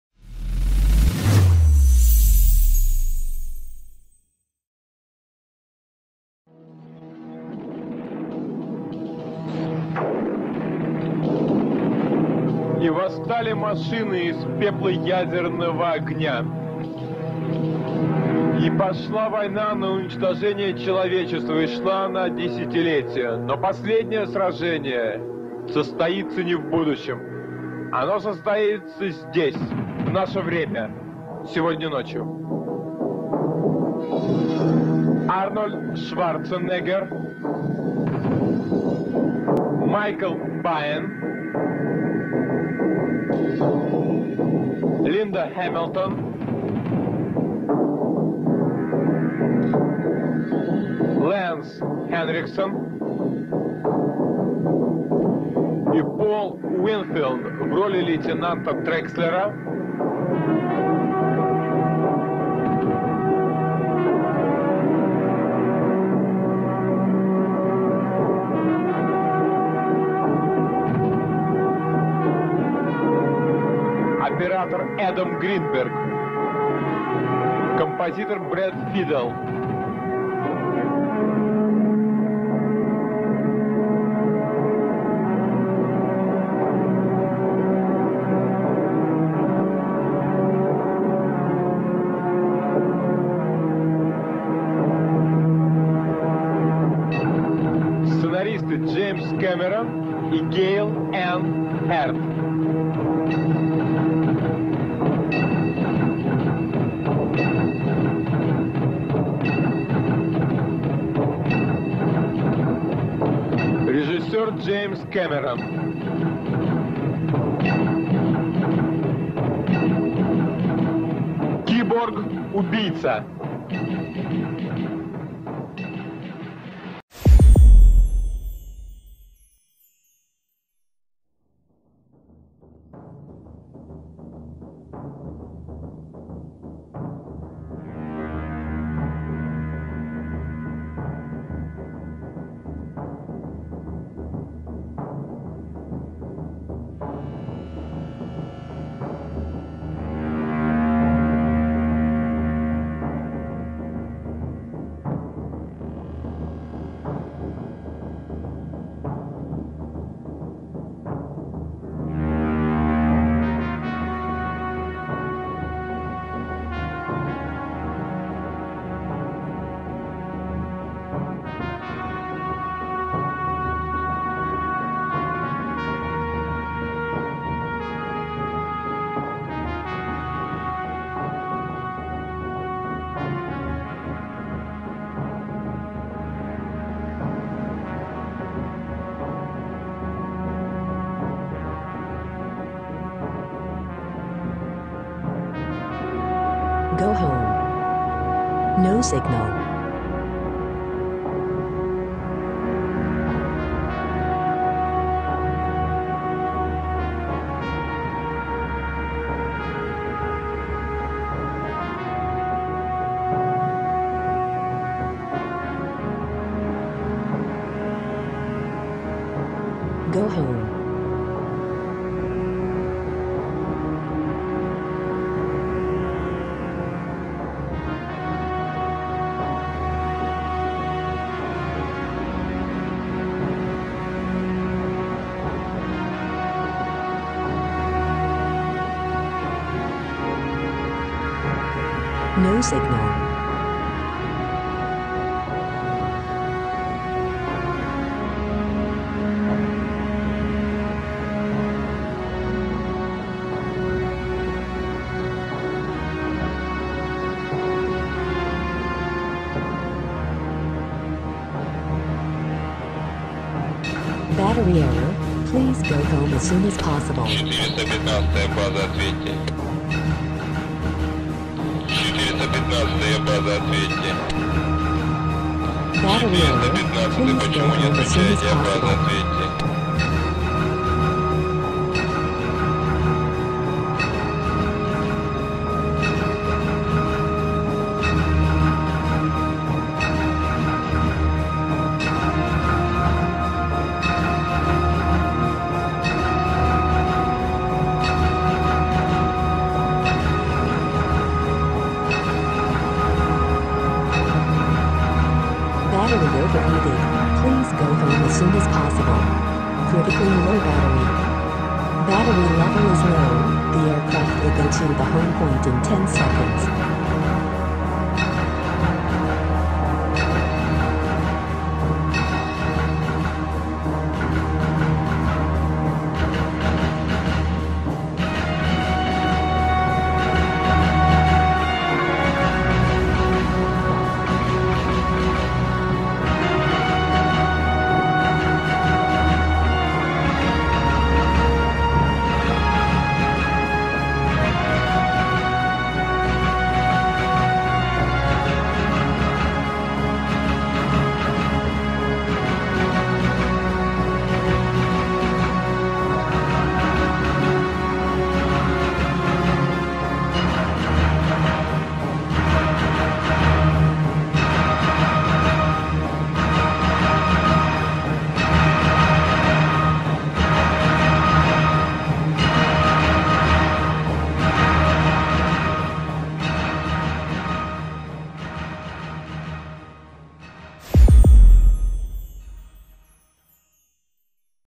🦾 Gomel dump in cyberpunk style
2143. A human colony reconnaissance drone detected a nest of cyborgs near Gomel.
Just looking at rubbish and landfill would be boring, so we did it.